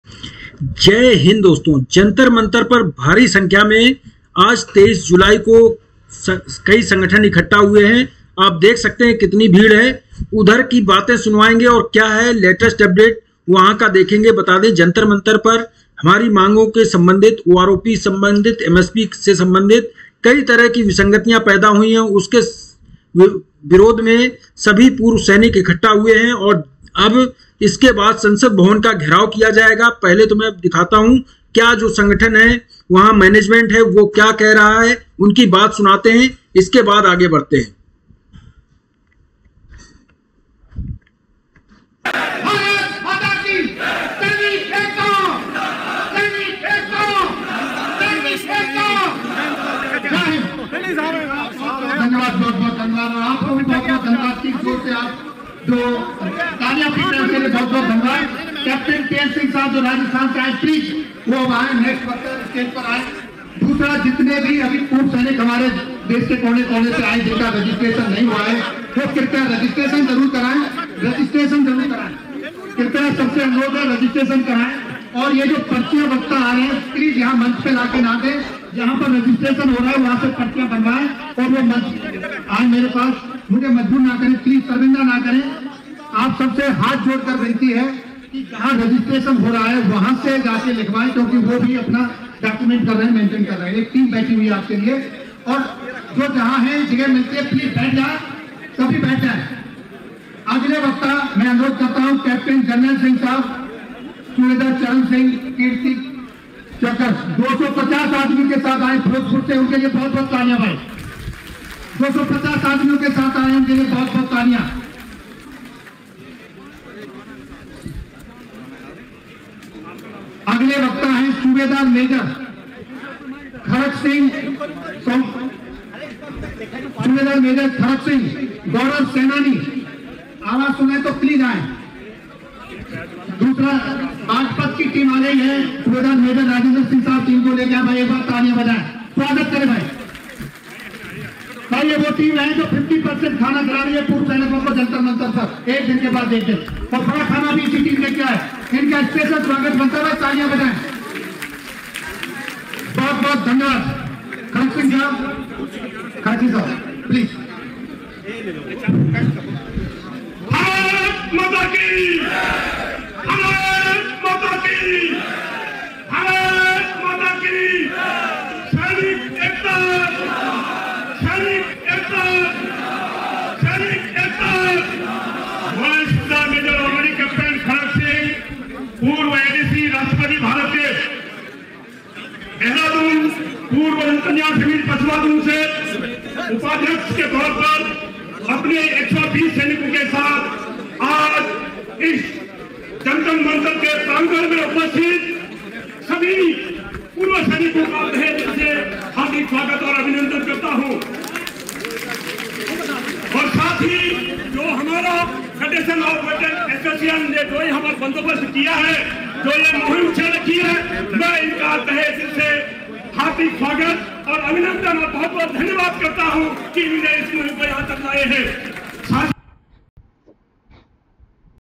जंतर मंतर-पूर्व सैनिकों का जमावड़ा देख मीडिया हुआ हैरान, संसद का घेराव होने जा रहा है , आगे पुलिस
IMPORTANT LINKS
NOTIFICATION-

100% EX- SERVICEMEN JOB/ ONLY FOR EX-SERVICEMEN JOB